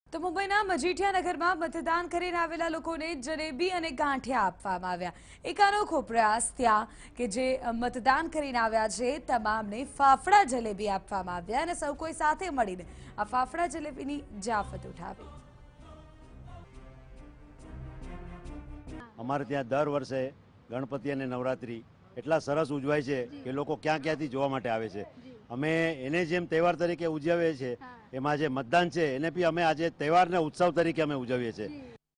LS Elections 2019: 'Jalebi-Fafda' given to voters of Majethia Nagar in Mumbai- Tv9
LS Elections 2019: 'Jalebi-Fafda' given to voters of Majethia Nagar in Mumbai